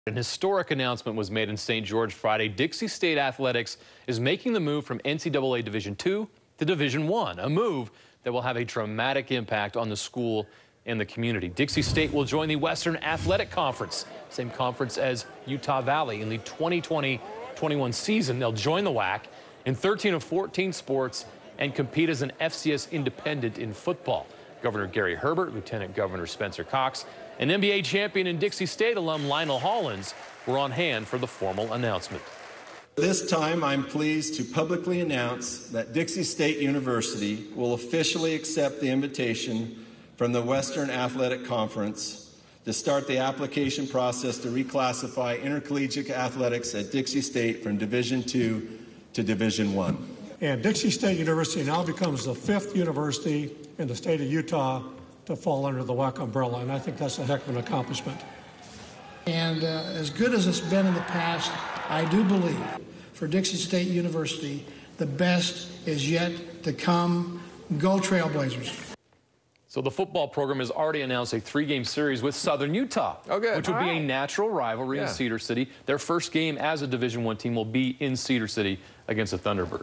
Dixie State announces move to Division I
KSL broke the news that Dixie State would join the WAC for the 2020-21 season and today, the University announced it had accepted an invitation to join the conference in all sports except football. The Trailblazers' football team will compete as an FCS Independent.